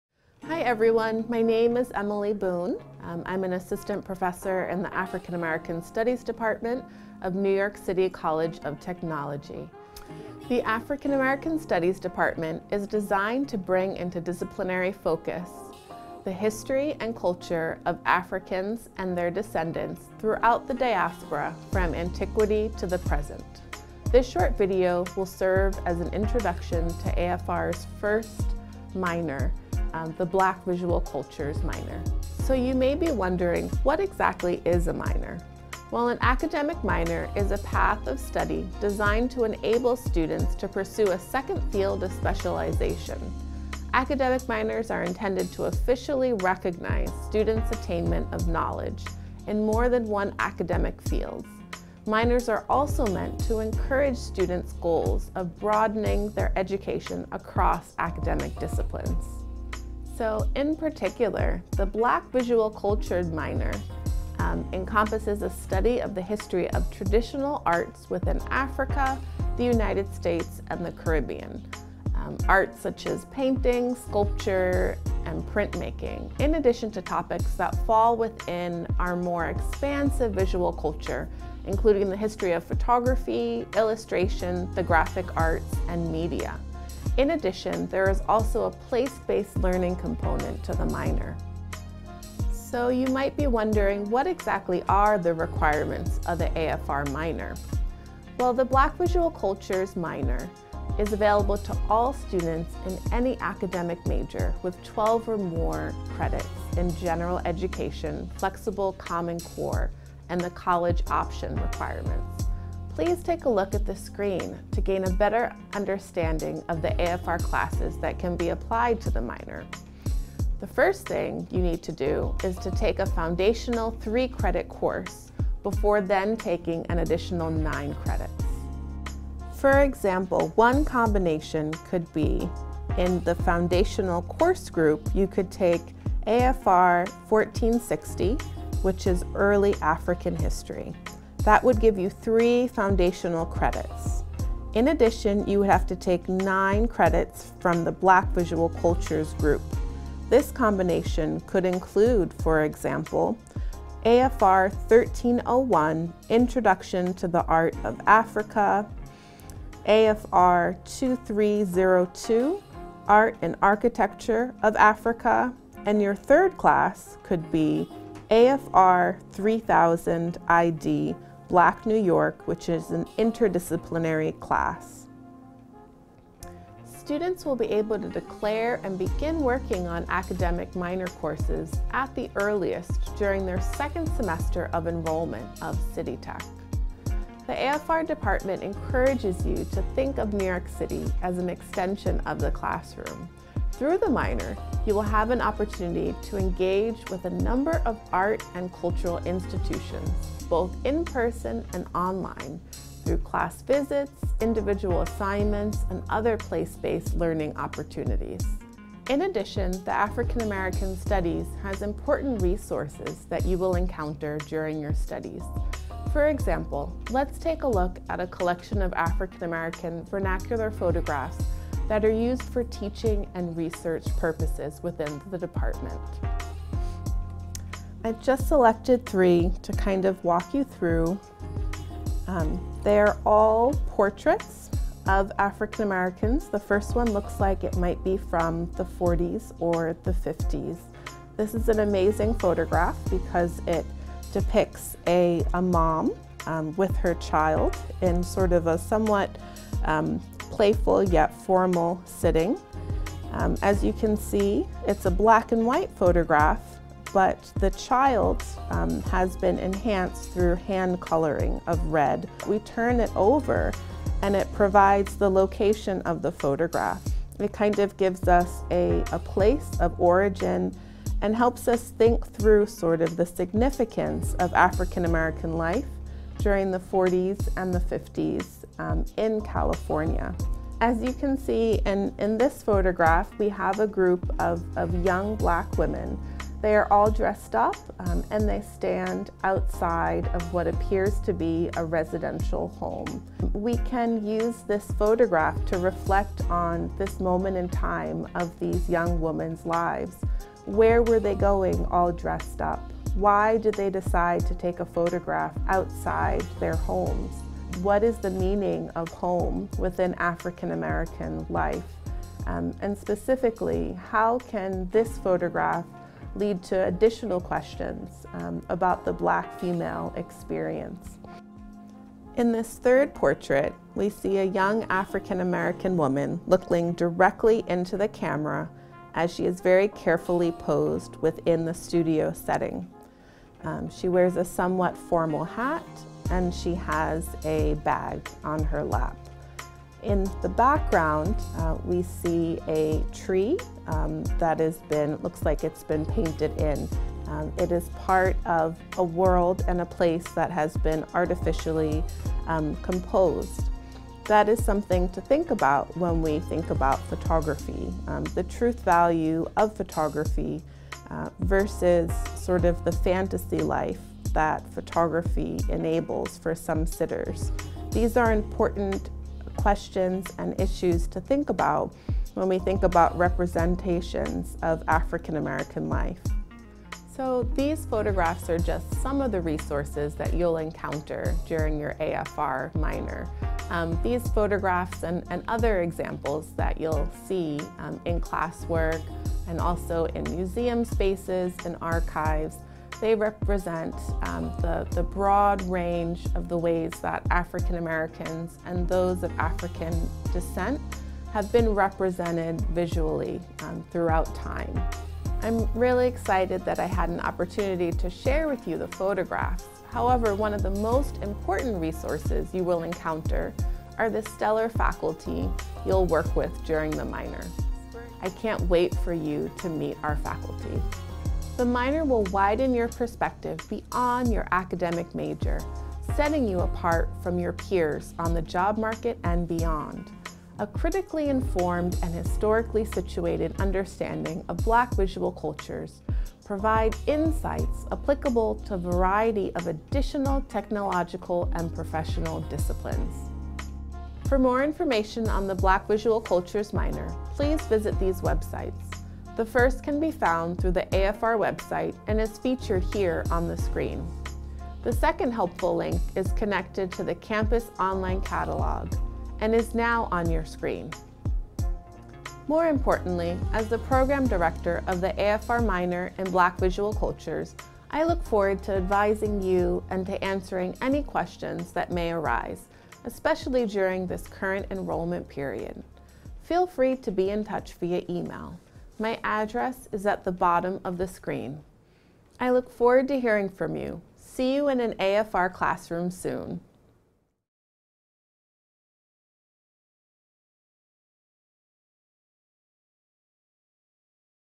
African American Studies Minor at NYC College of Technology, CUNY
The Black Visual Cultures Minor . . . is available to all students in any academic major with 12 or more credits in General Education Flexible Common Core and the College Option Requirements. Students will be able to declare and begin working on academic minor courses, at the earliest, during their second semester of enrollment at City Tech.